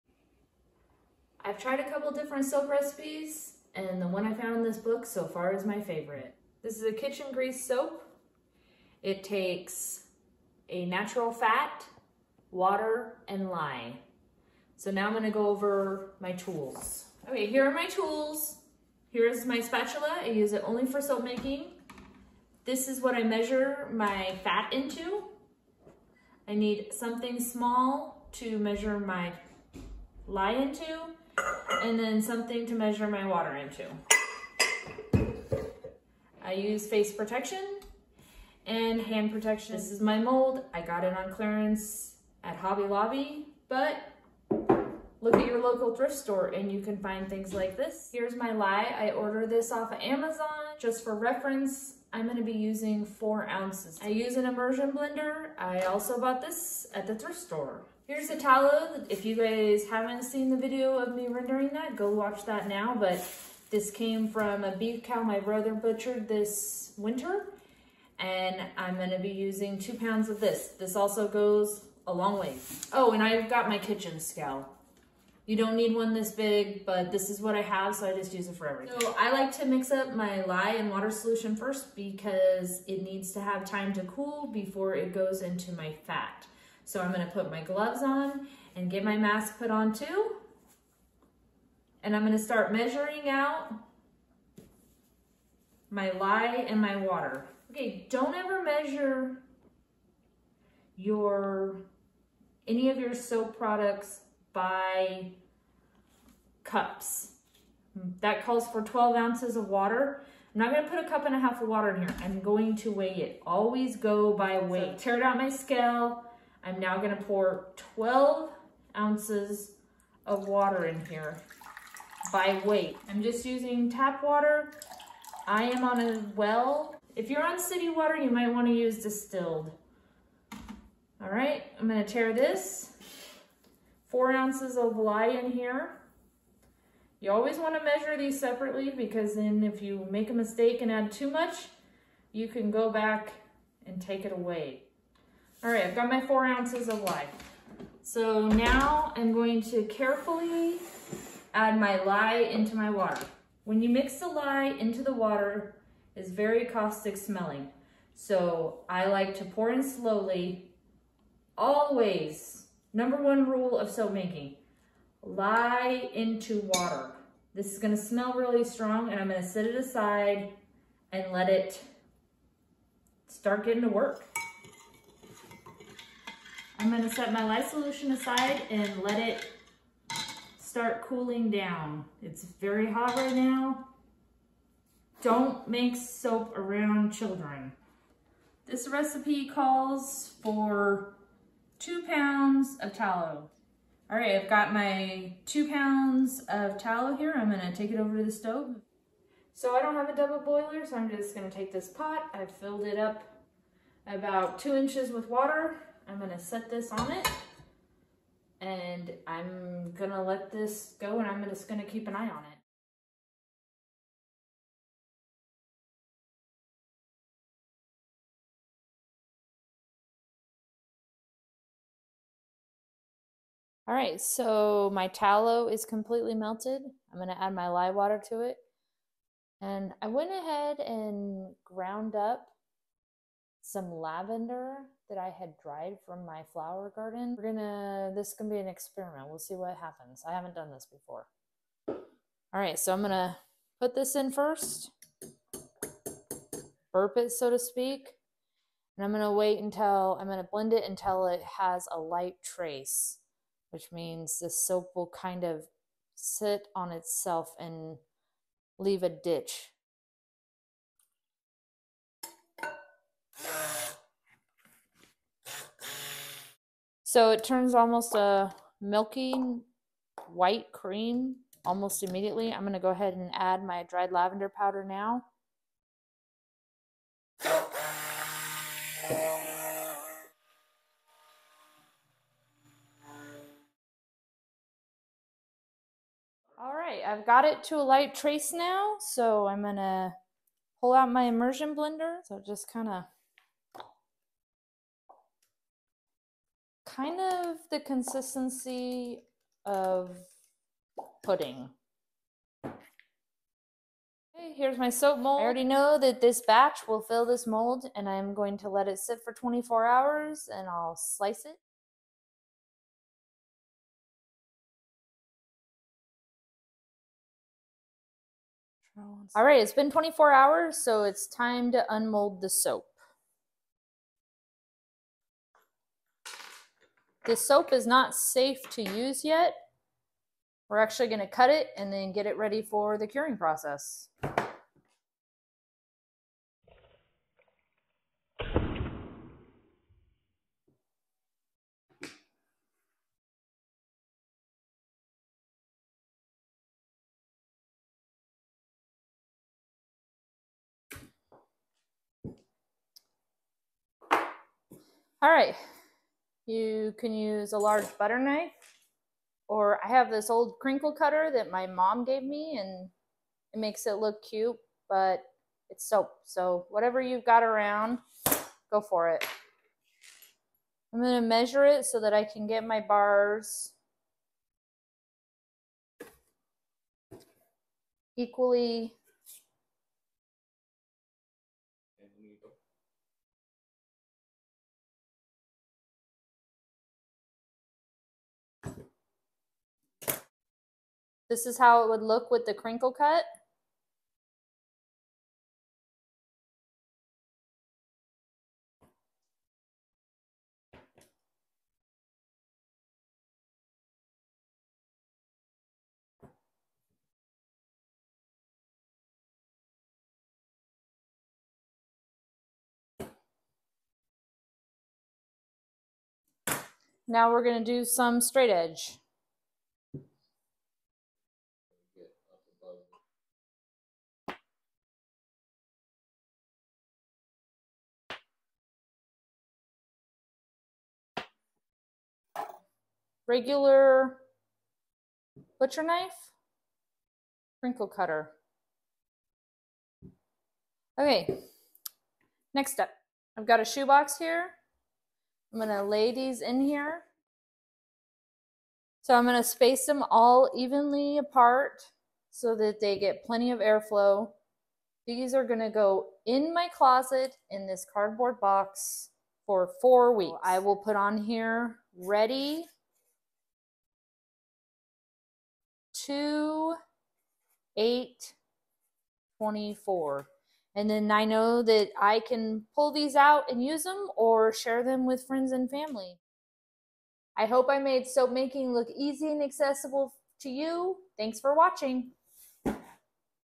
Simple Soap to Make at Home
My wife has been making her own soap for years now. After many different experiments, and recipes she finally landed on one that she calls her favorite. Throughout the years she’s made the decision based on how the soap actually reacts to her sensitive skin and what it towed to make it. We hope you enjoy the process and the results of your experiments. We really appreciate your time.